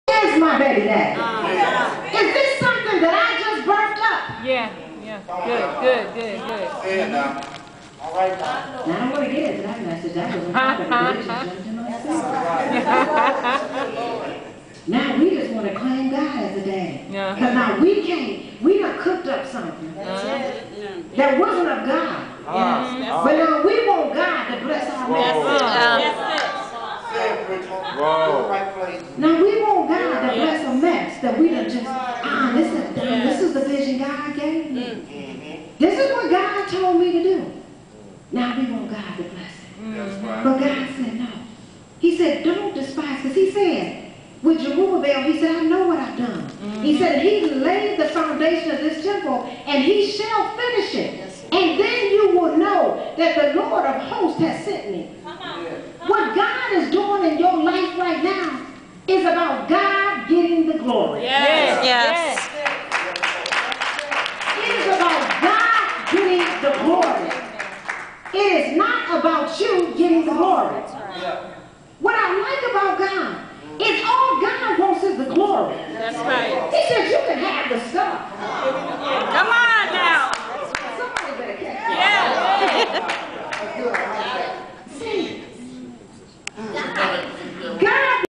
Bless my mess
This is a video of Norma Washington at the "She Shall Be Called Woman" conference in Mesa, AZ, on October 31, 2010, where I spoke this weekend. She is sharing on how we come up with our vision for ourselves, then create messes and expect God to bless it.